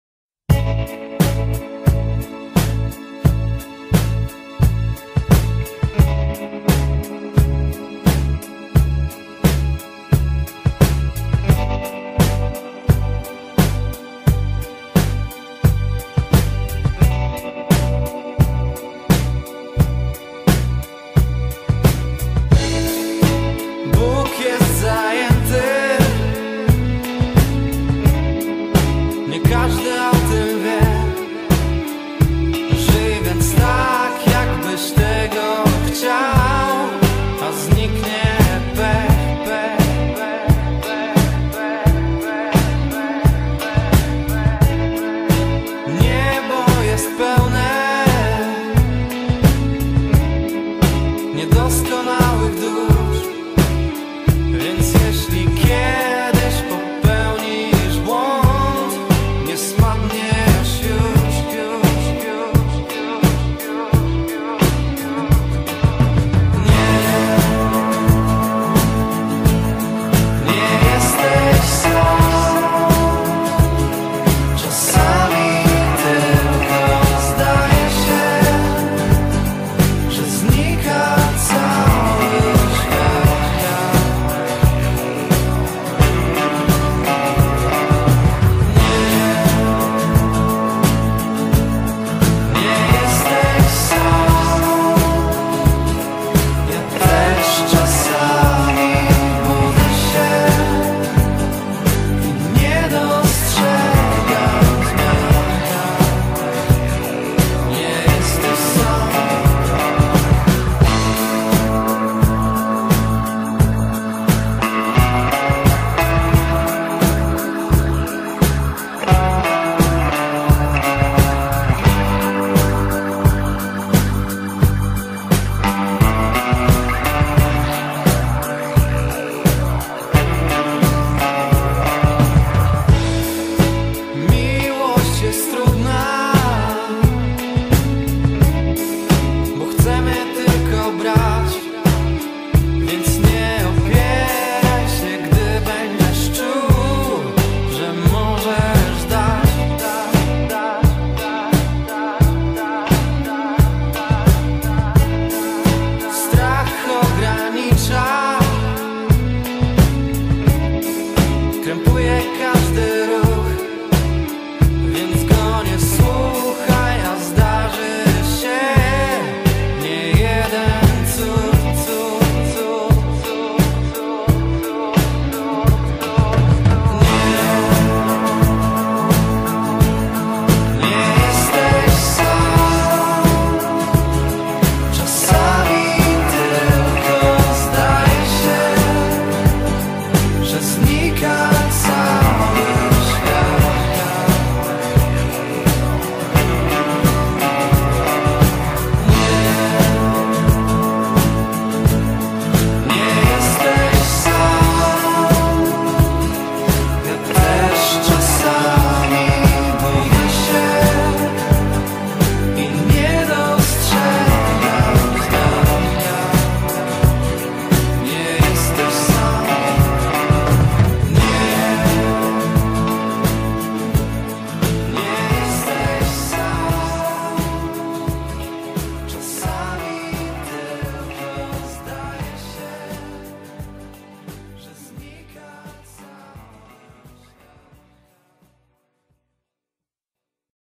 Basset - Nie jesteś sam .
Wiele Basset'ów jest bitych i w inny sposób krzywdzonych przez swoich właścicieli, powinniśmy im pomóc zapomnieć o tym co była i dać im nową i kochającą rodzinę. Pomogą nam strony i fora takie jak ;
Zapraszam i dziękuję za uwagę. ;)